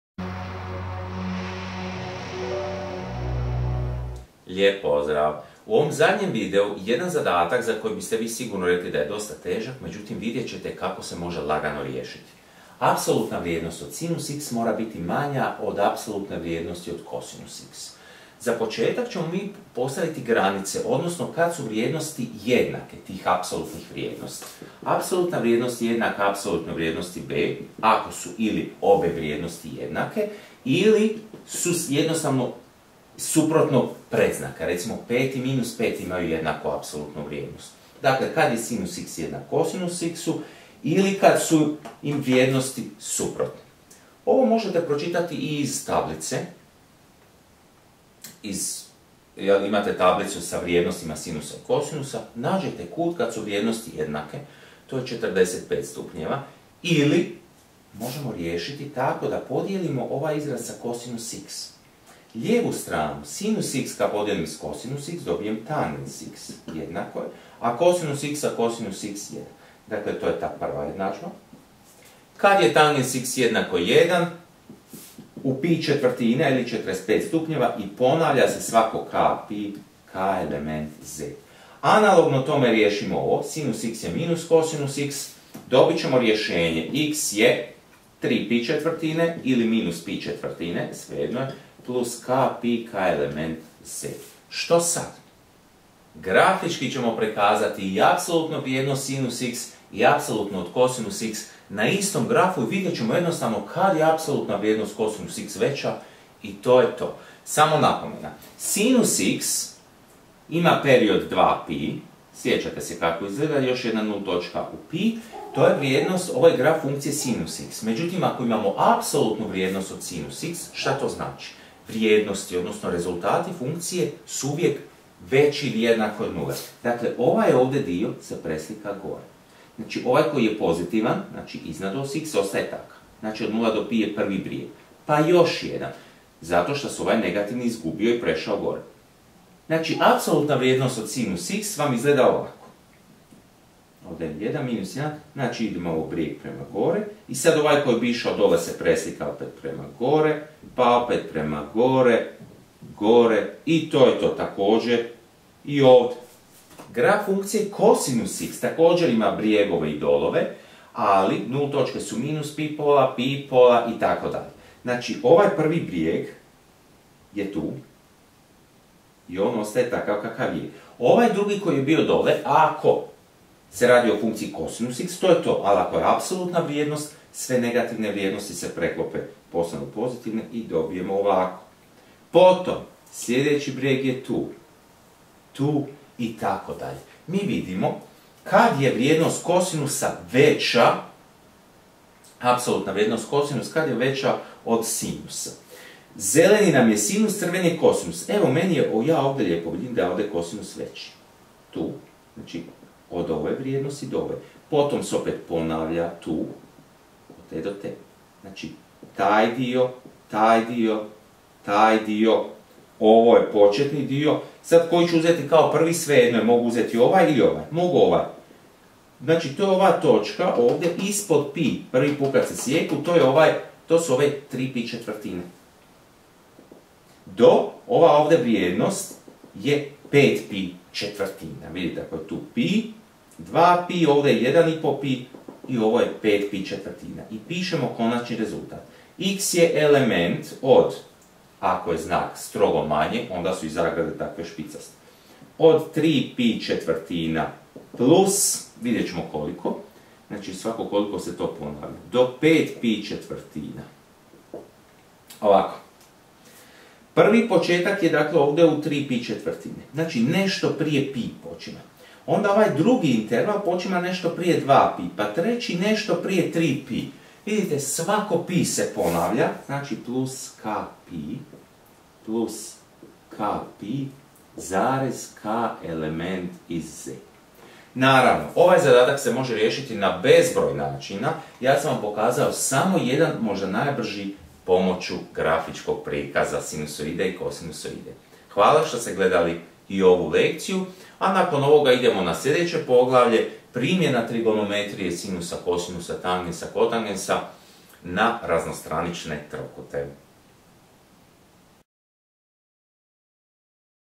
Trigonometrijske nejednadžbe 09
Riješite sljedeću trigonometrijsku nejednadžbu:
str. 125, Matematika 3, 1. dio autori: Dakić, Elezović